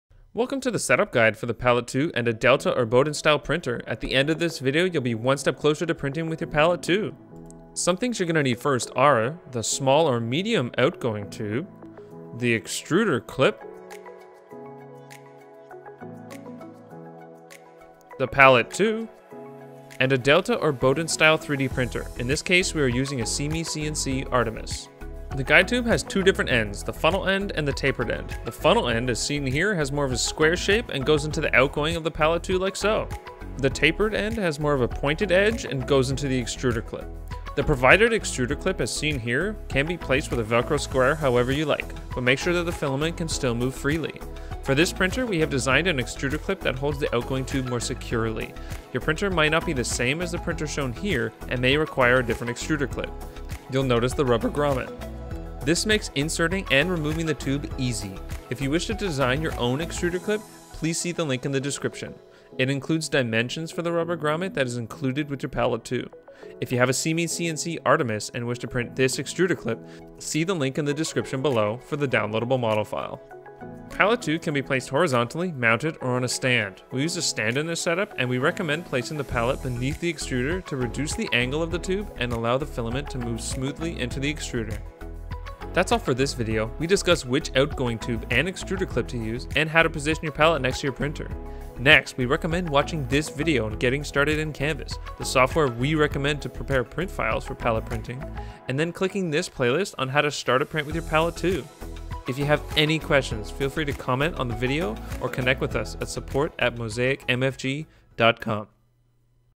Setup Guide: Delta/Bowden Printer (SeeMeCNC Artemis) with Palette 2™
This video demonstrates how to get started with Palette 2 and a delta/bowden printer in this case we demonstrate the setup on a SeeMeCNC Artemis. Palette 2 upgrades 3D printers to print in multiple colors and materials.